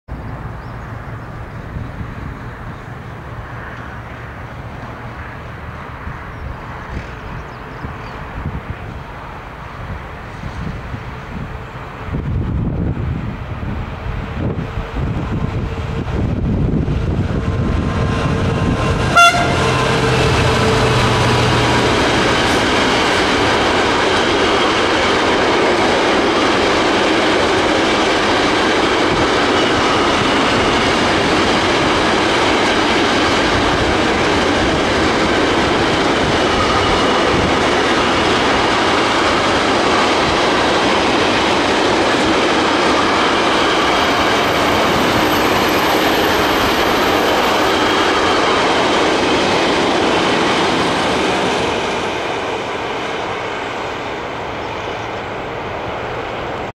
ERS Railways class66 diesel locomotive with container train+ Trainhorn action!!!
At Blerick,the Netherlands ,April 1 2010